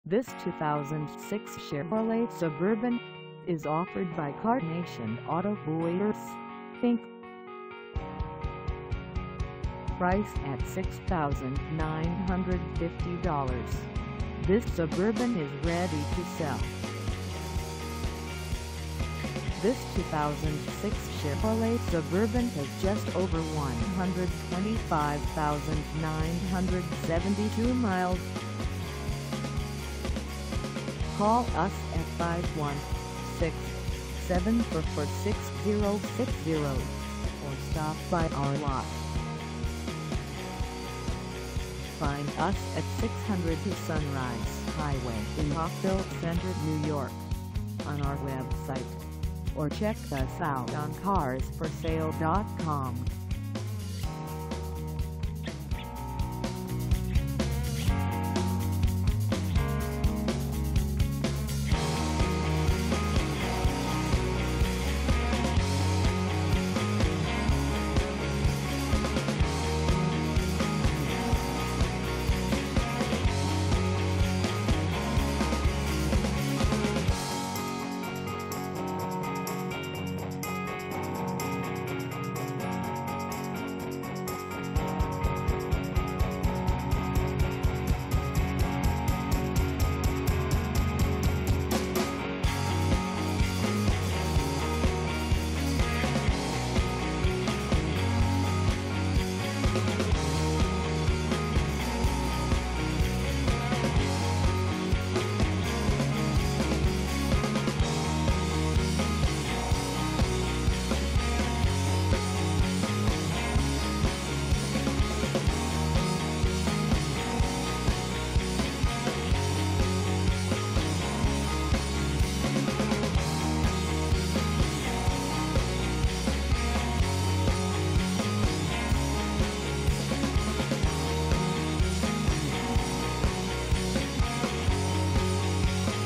2006 Chevrolet Suburban LT 1500 4dr SUV 4WD for sale in Rock
This 2006 Chevrolet Suburban LT 1500 4dr SUV 4WD is for sale in Rockville Centre , NY 11570 at CarNation AUTOBUYERS, Inc..

Engine: 5.3L V8
Transmission: Automatic 4-Speed
Color: Champagne
Mileage: 125972
Contact CarNation AUTOBUYERS, Inc. 602 Sunrise Highway  Rockville Centre , NY 11570 to test drive this 2006 Chevrolet Suburban LT 1500 4dr SUV 4WD today.